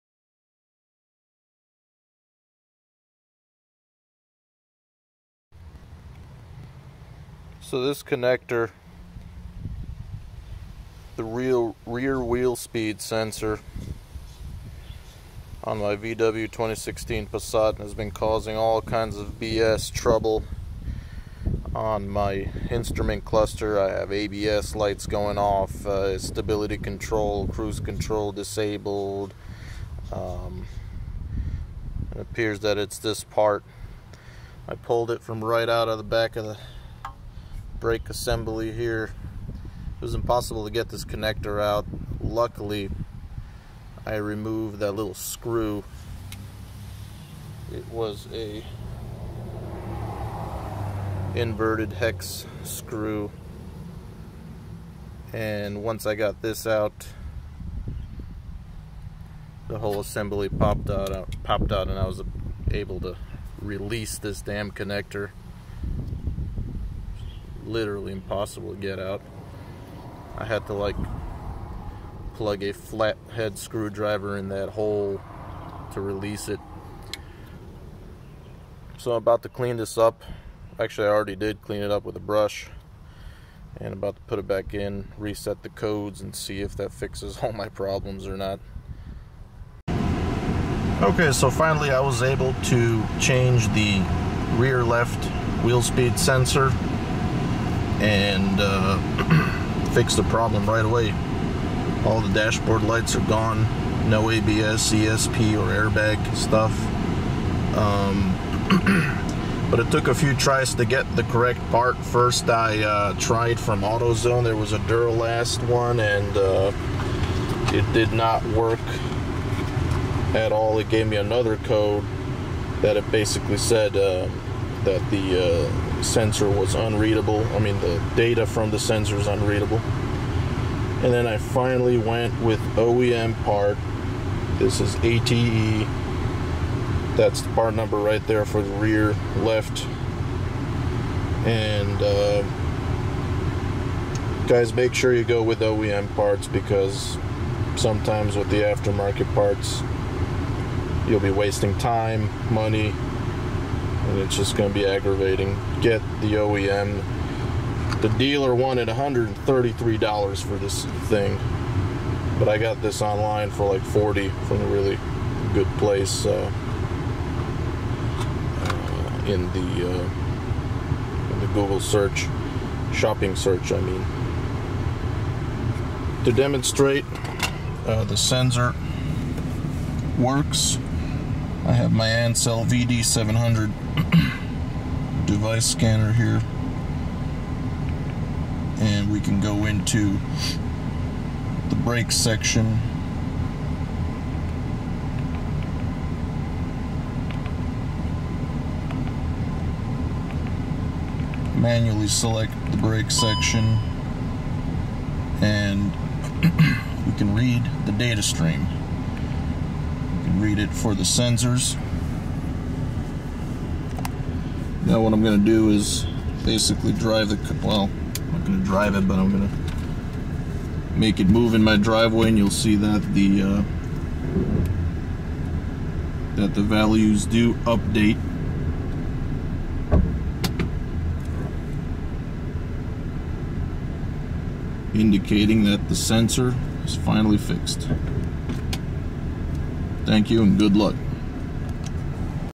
VW Passat 2016: Replacing the ABS wheel speed sensor
Airbag, ABS, ESP lights on? Check your wheel speed sensors!

Update: After about 10 months, my rear RIGHT sensor has gone faulty now. I don't know what it is with these cars and these sensors. I went ahead and replaced it. Tip: the screw is a T30 male torx. It was hard to remove even with the screw out. I pryed it off with a screwdriver gently. Replacement works well.